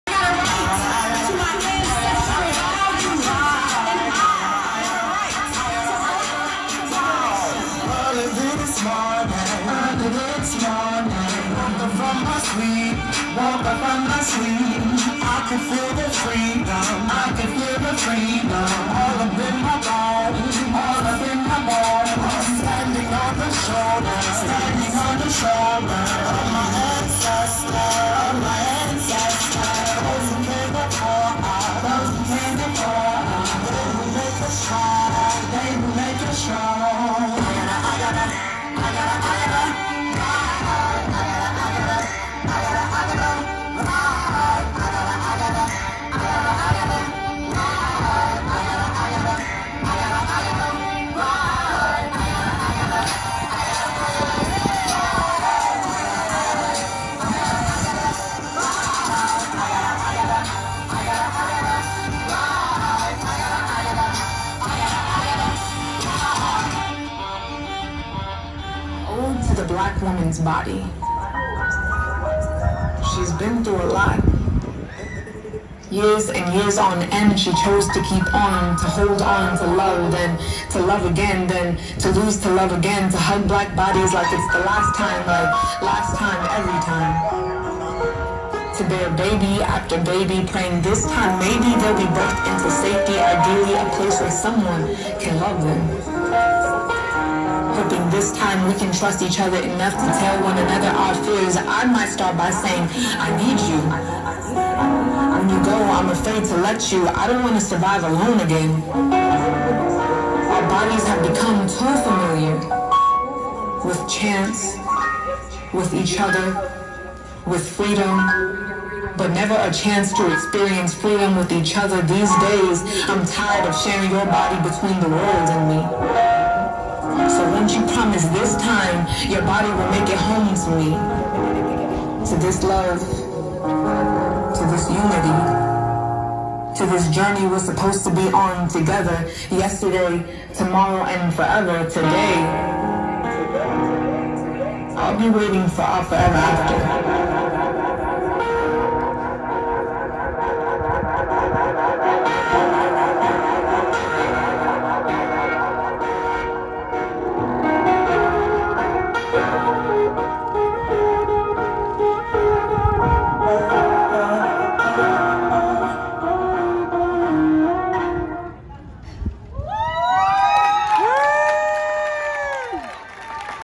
Women’s March Dance
Dance team dances for love and unity